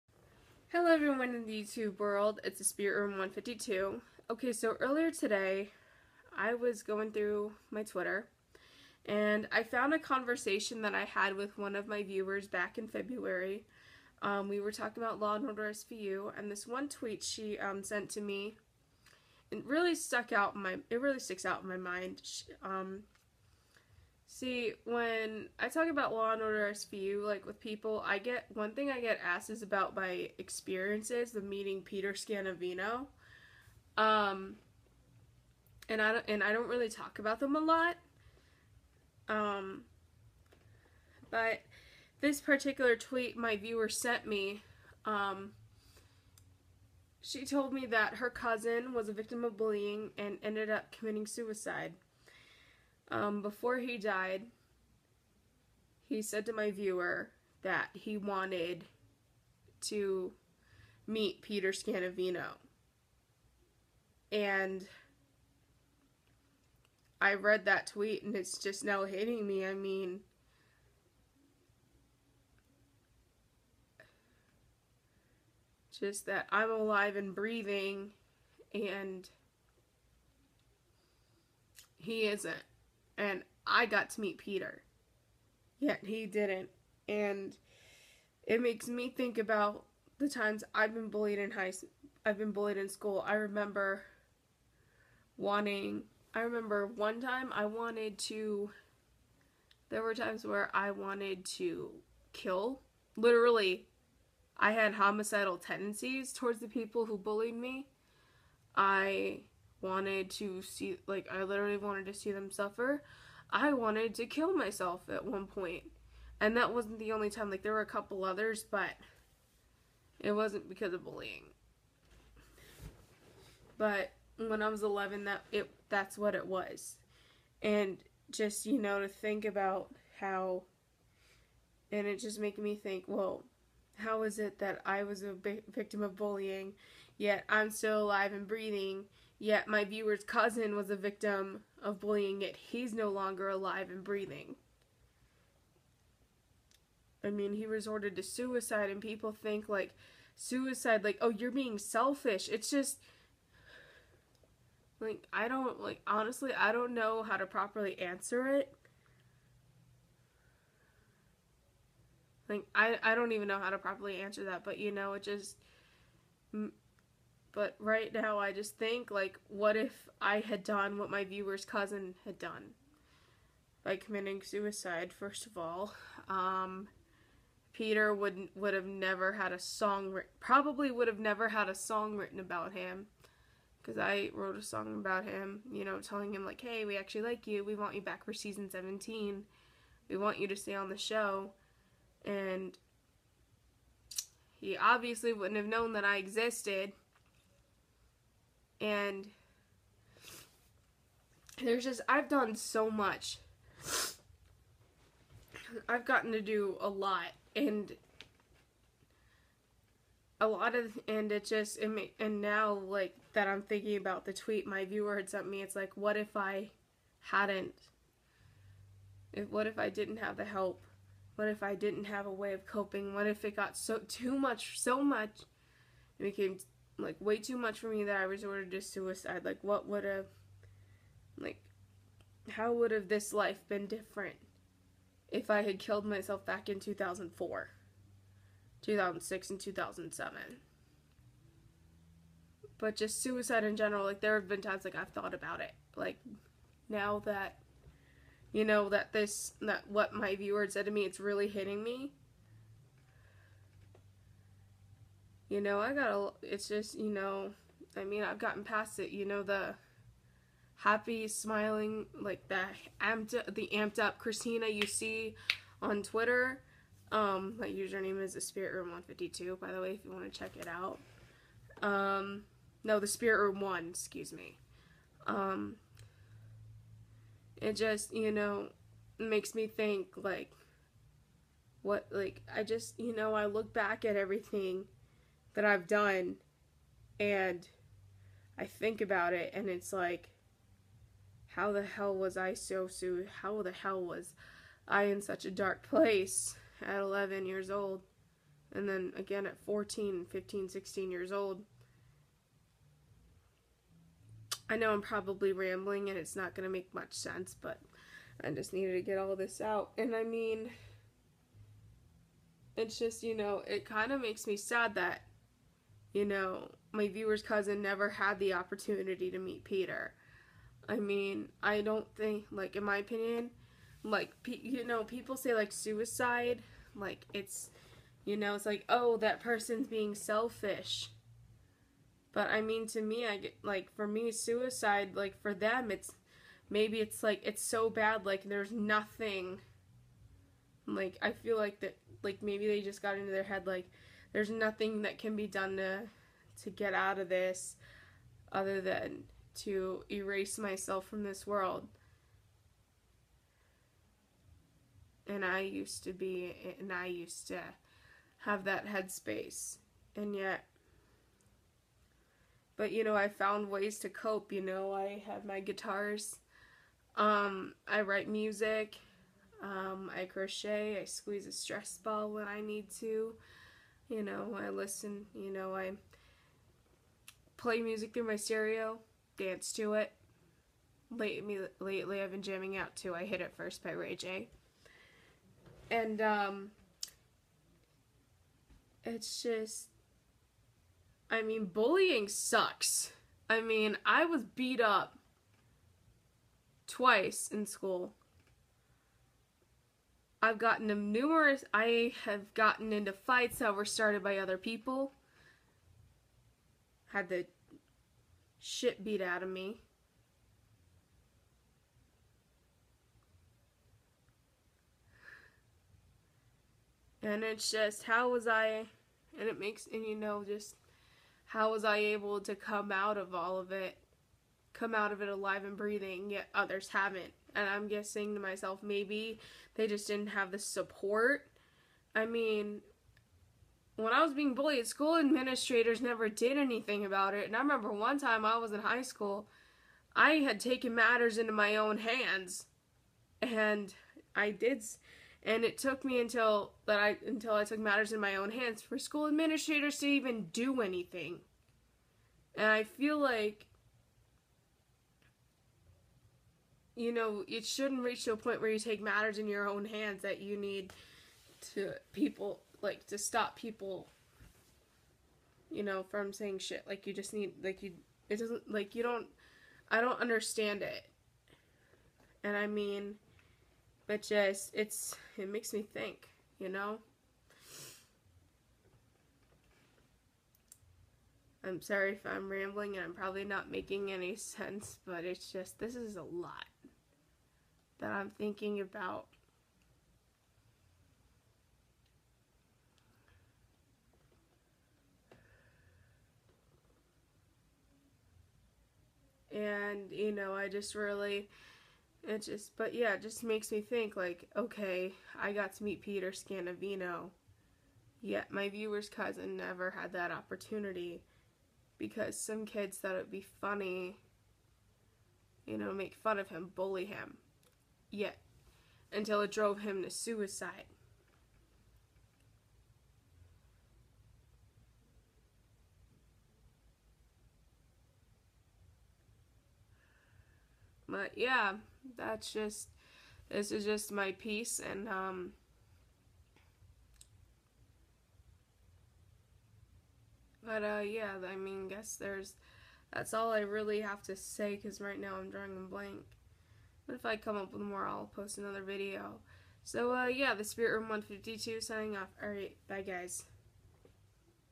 A viewer's tweet hit me hard emotionally and this is my response to it
Trigger warning: Bullying, suicide

Excuse my rambling. I apologize if this video is out of order and if some of it is repetitive. This was an emotional video for me to make.